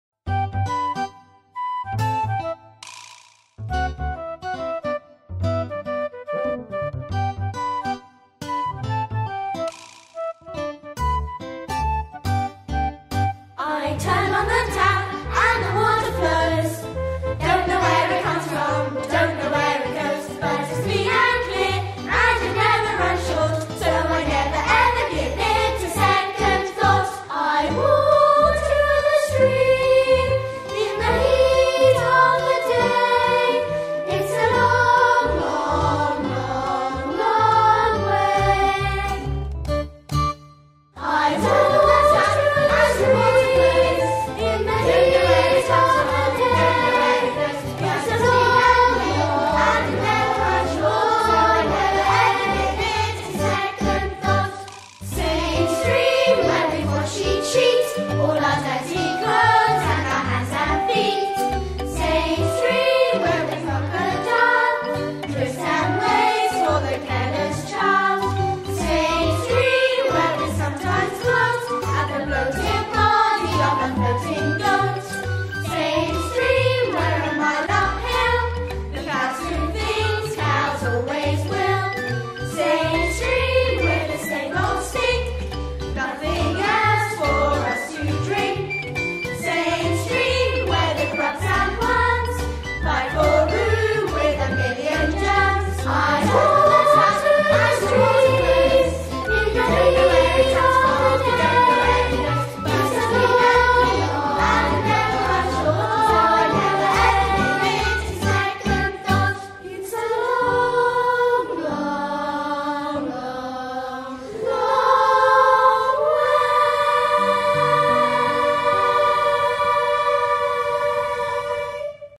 Holy Trinity Primary School, Bradley Stoke, Year 5 animation
The Year 5 Class at Holy Trinity Primary School in Bradley Stoke created this plasticine animation video for their school assembly.